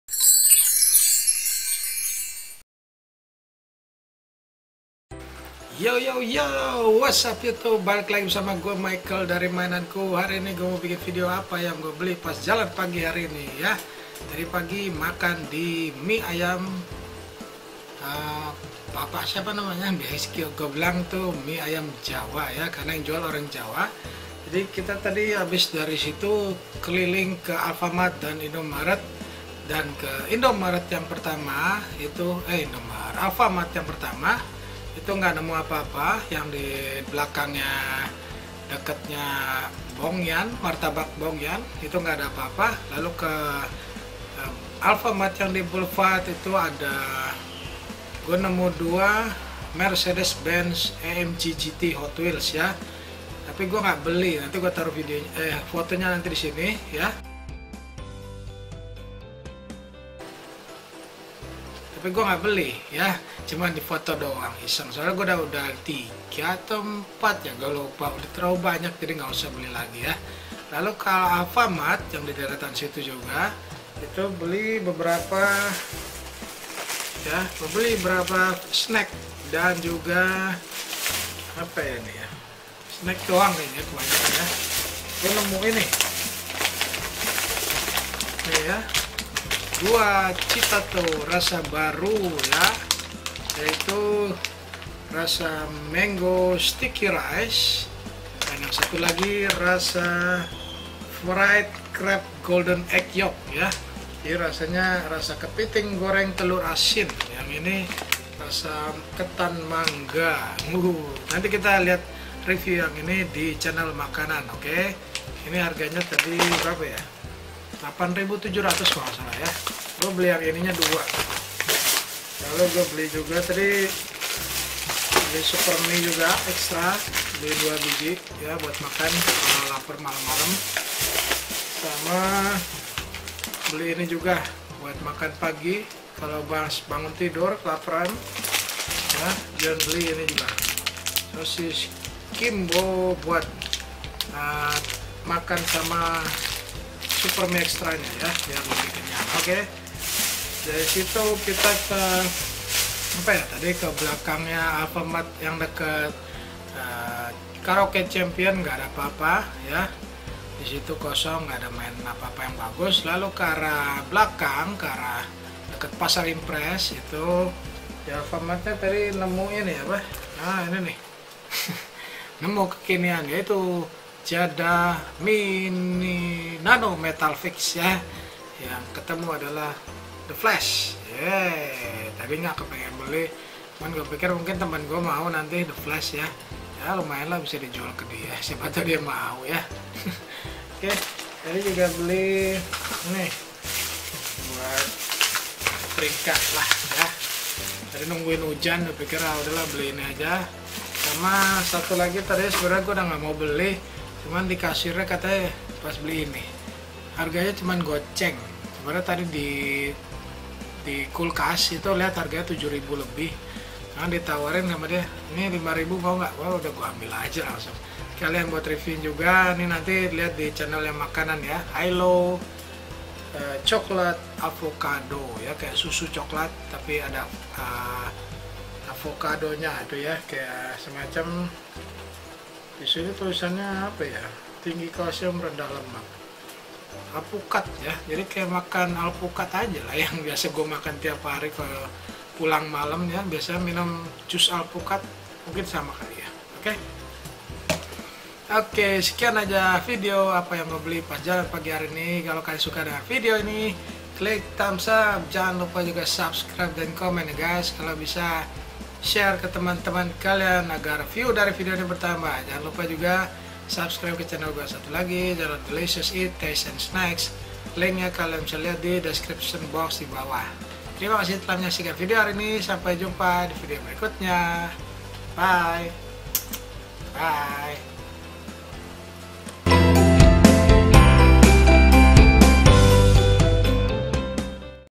Barang Yang Gw Beli Pas Jalan Pagi 15
Kemarin jalan pagi seperti biasa keliling kelapa gading mampir ke bbrp alfamart & indomaret & ini video brg yg gw dpt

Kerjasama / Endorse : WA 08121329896